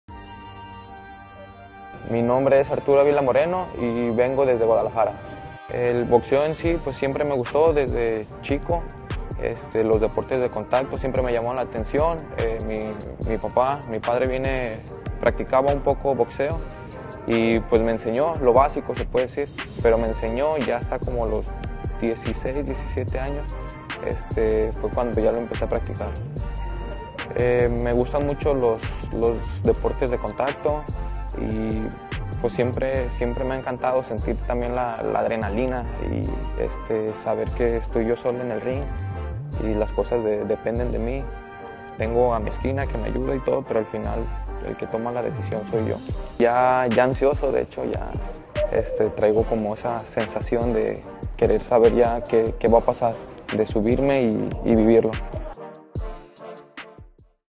Arturo Avila Moreno - Boxfest III Teaser - Presented by Red Owl Boxing
Mathusan Mahindas (4-0-0) vs Arturo Avila Moreno (2-0-1)

March 11th, 2023 Live from The Owl's Nest

Red Owl Boxing Brampton
25 Kings Cross Rd.
Unit 5A
Brampton, ON L6T 3V5